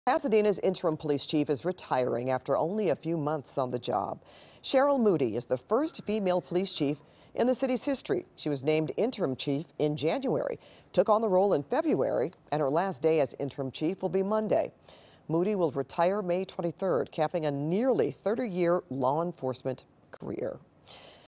Pasadena's first female police chief retiring
Pasadena's first female police chief Cheryl Moody said she will retire on May 23rd after a nearly 30-year law enforcement career.